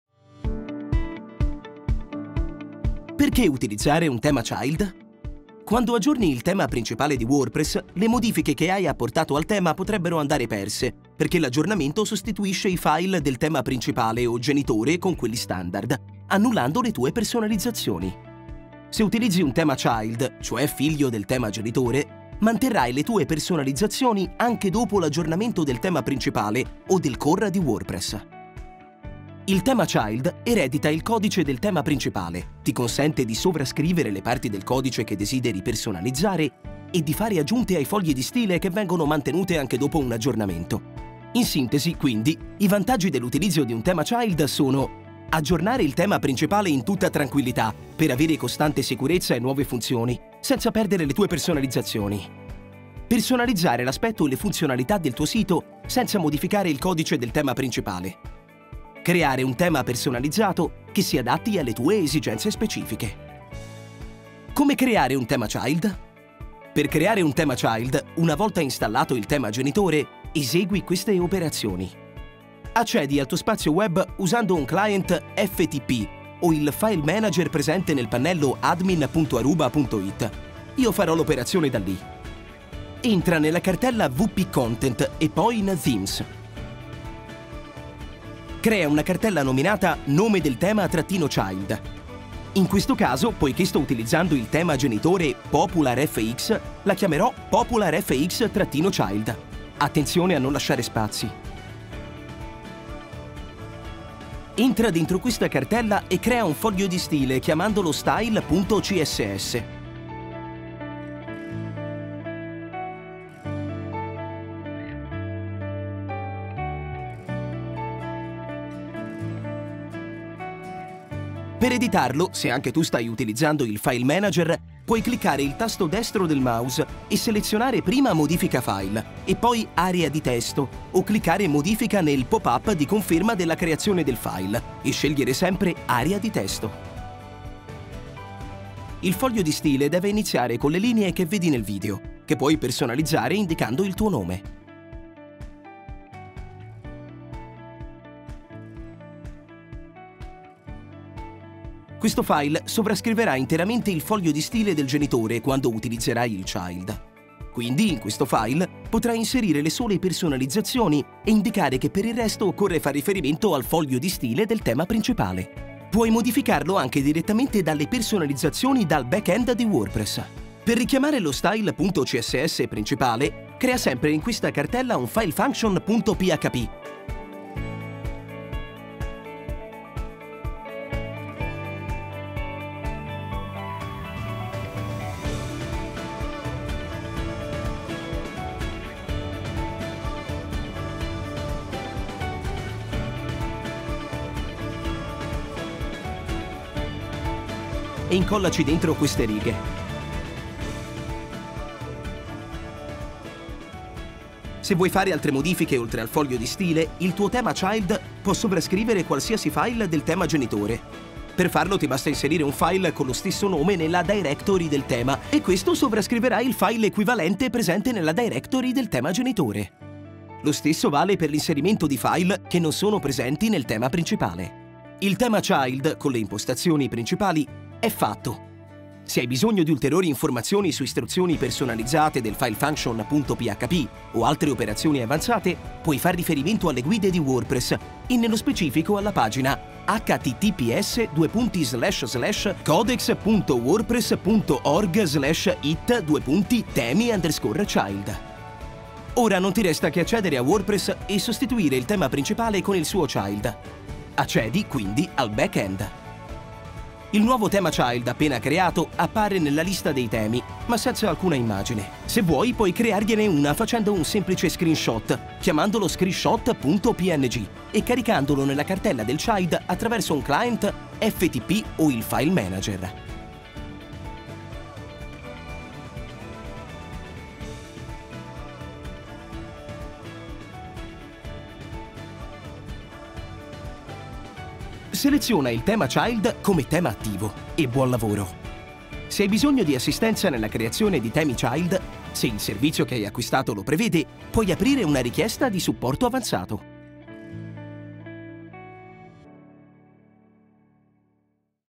WordPress - installare e gestire tema child
Perché e come usare i temi child di WordPress per mantenere le personalizzazioni del tuo sito anche dopo gli aggiornamenti.